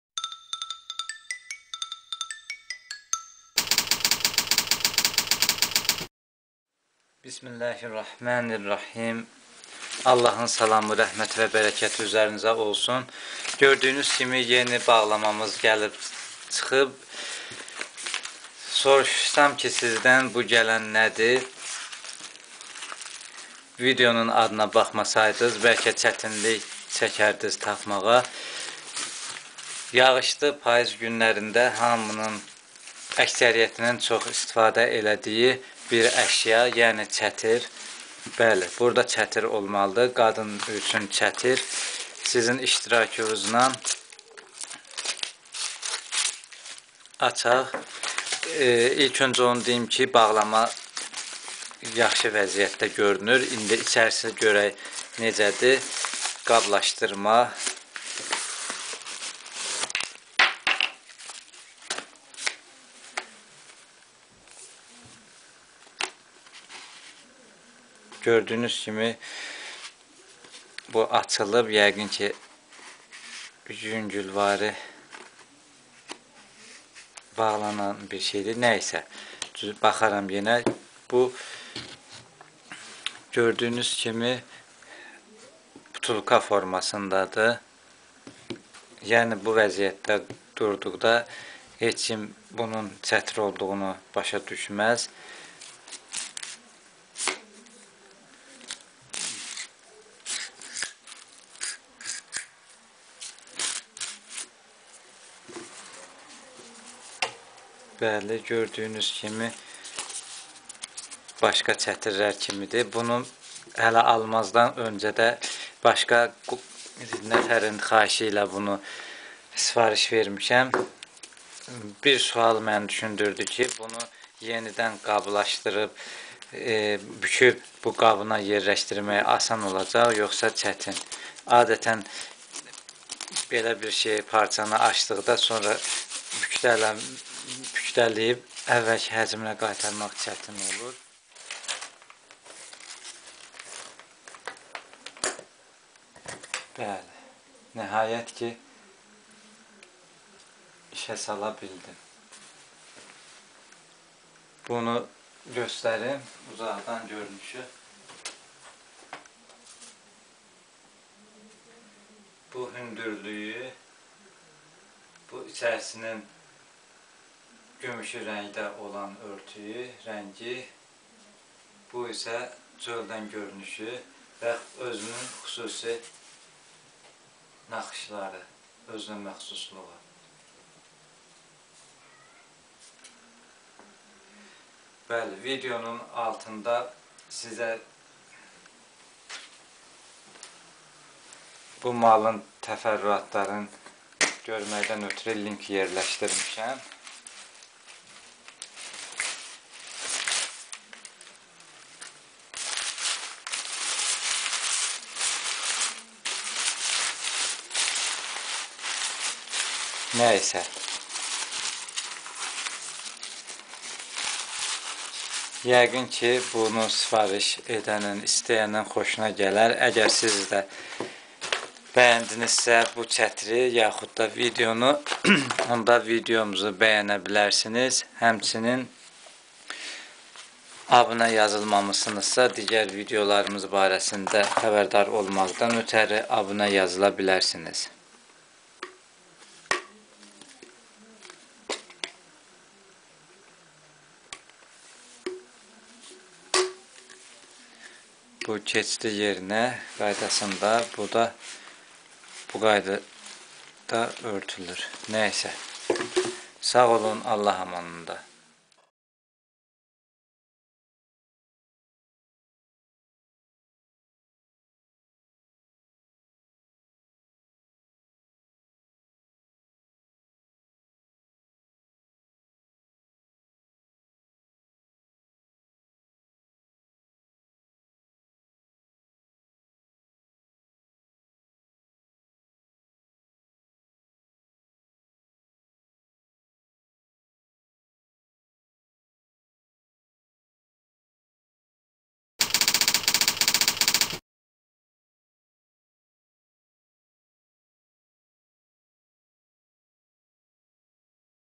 Çətir (qadınçün) - Ebay.com-dan bağlama #7
Qara rəngli, butulka dizaynında qadın yağış çətiri
Ⓕ Facebook səhifəmiz: